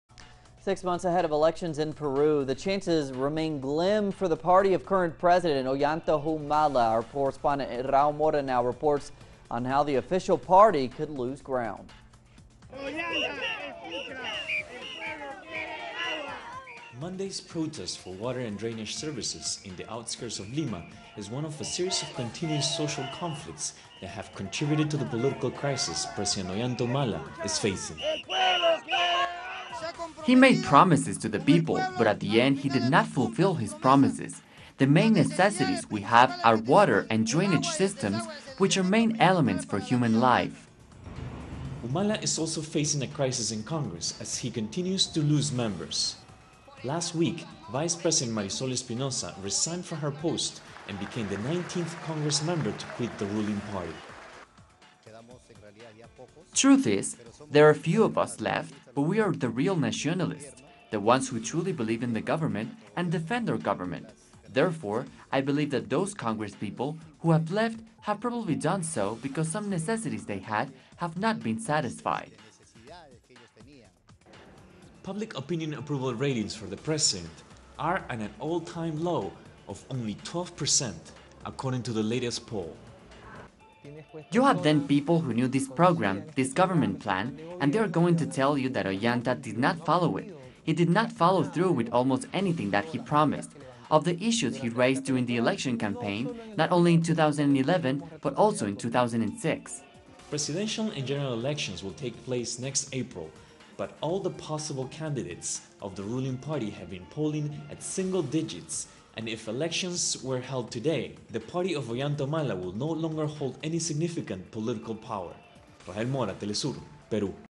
Peru: Humala Approval Ratings Plummet to 12%
The series of social conflicts that have rocked Peru in recent months coupled with unfulfilled campaign promises have weakened support for President Ollanta Humala, whose approval ratings have plummeted to 12%. Humala's Peruvian Nationalist Party (PNP) could face an uphill battle in next April's elections, as its possible candidates have single-digit approval ratings. Humala also faces a crisis in Congress, where vice-president Marisol Espinoza resigned from the PNP, the 19th lawmaker to do so. Rael Mora reports from Lima, teleSUR